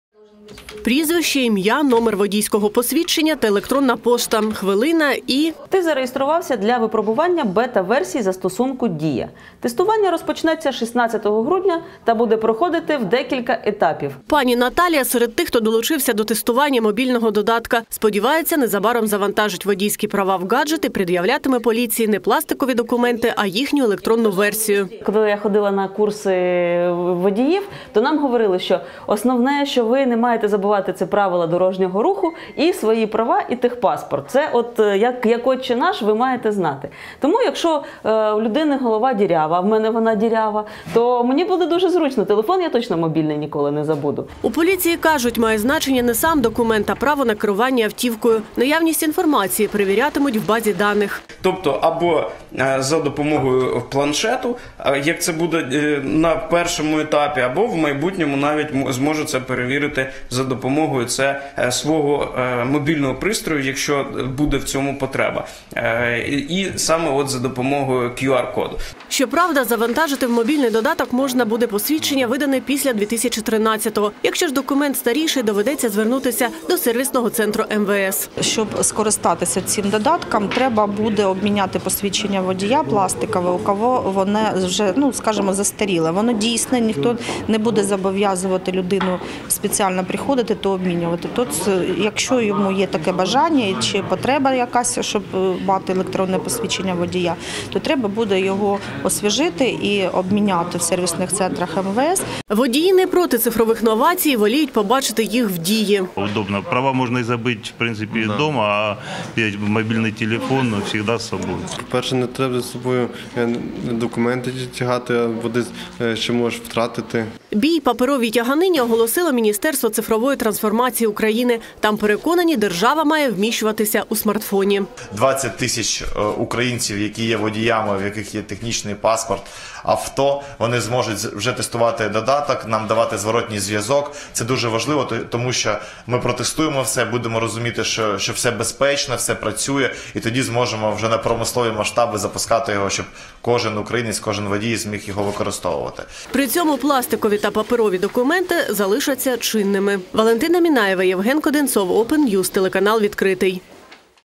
Водійське посвідчення у смартфоні: в Україні почалося тестування мобільного додатка
Держава у смартфоні. Забув вдома посвідчення водія чи техпаспорт – скоро це буде не проблема. Дістав мобільний – і все там, у додатку «Дія». Сьогодні стартувало тестування бета-версії застосунку. Долучитися до проєкту міг кожен бажаючий. Подала заявку і знімальна група Відкритого.